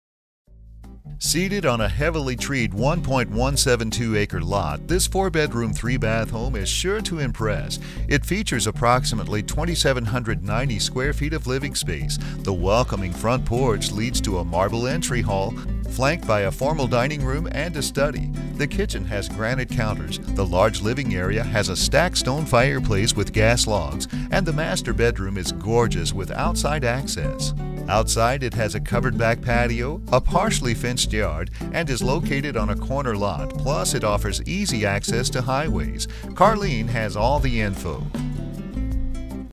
Home for sale in Boerne, TX | $347,900
4BR, 3BA home approx. 2790 sq ft. on heavily treed 1.172 acres. Welcoming front porch leads to marble entry hall flanked by formal dining and study. Granite counters in kitchen, built in oven and five burner gas cook top. Nice master down with separate tub/shower, with outside entry. Large living w/stacked stone fireplace w/gas logs. Fourth bedroom could be gameroom. Large covered back patio. Partially fenced yard. Located on corner lot. Easy access to highways.